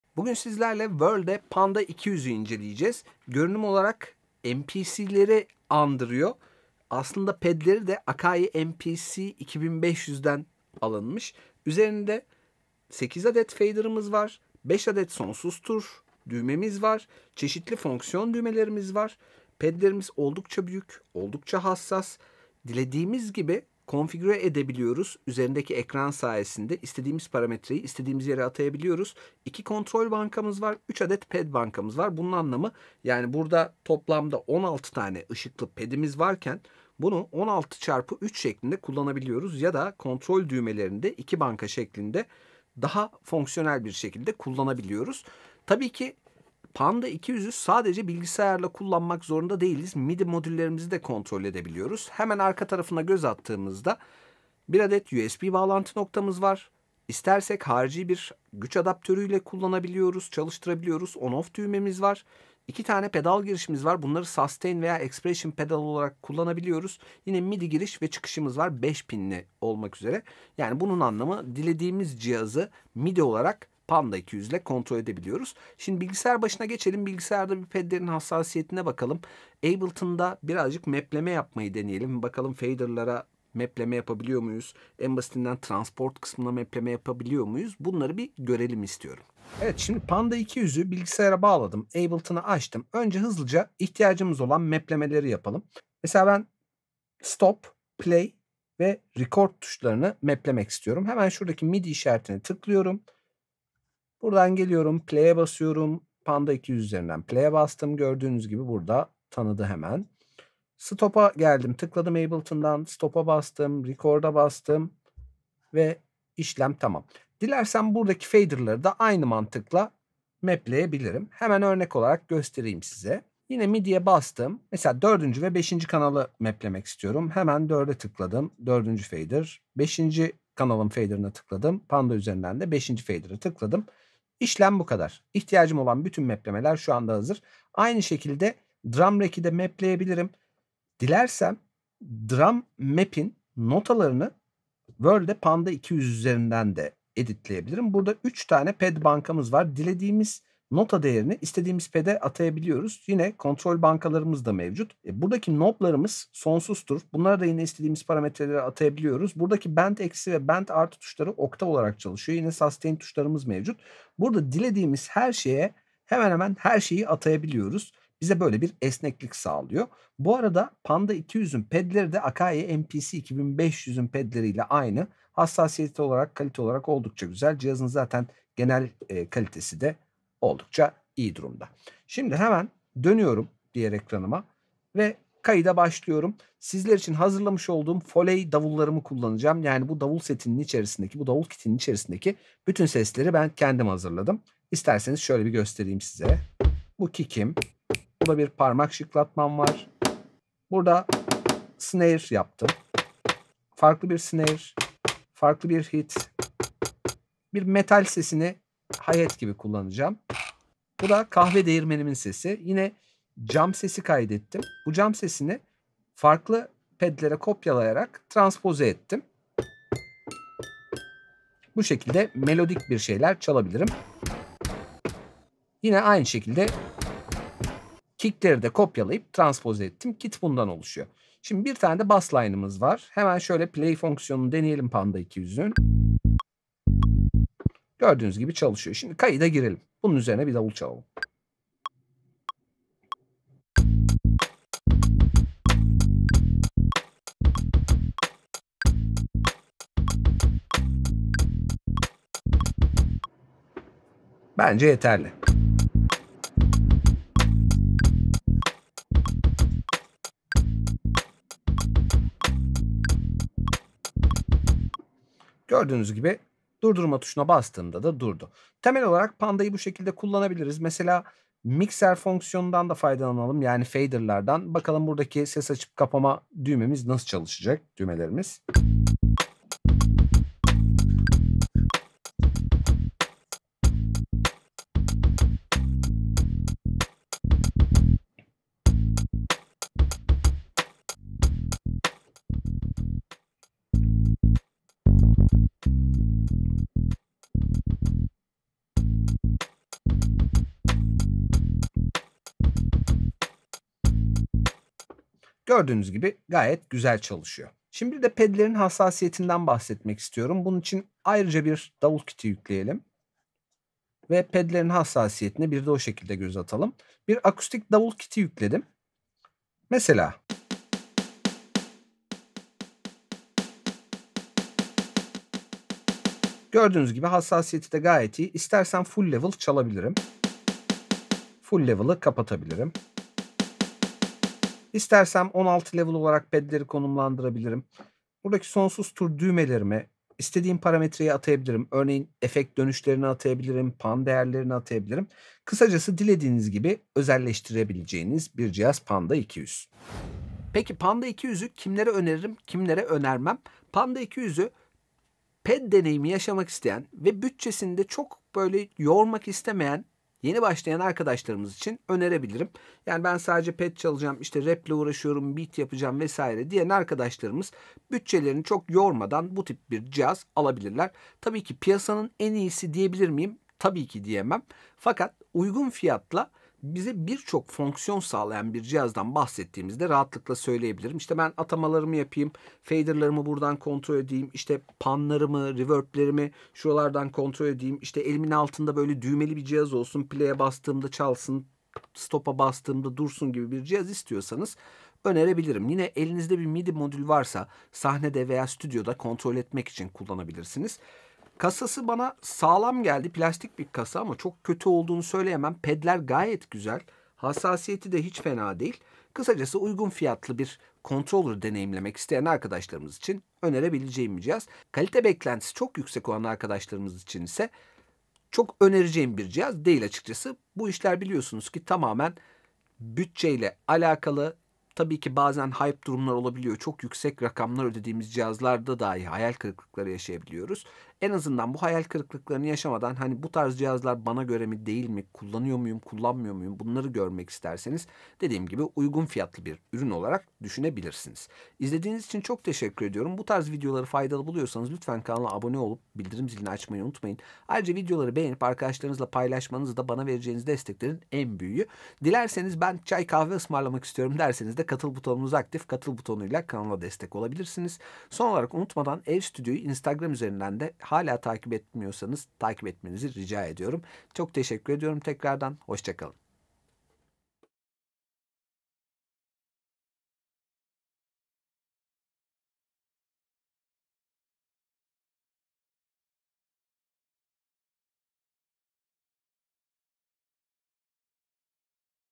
WORLDE PANDA 200 MIDI PAD CONTROLLER İNCELEMESİ
MPC tadında fakat uygun fiyatlı bir controller arayan arkadaşlarımız için Worlde Panda 200 midi pad controller 'ı inceledim. Ürün linklerini aşağıda bulabilirsiniz. Keyifli seyirler...

Müzik Programları (DAW Seçimi vb.)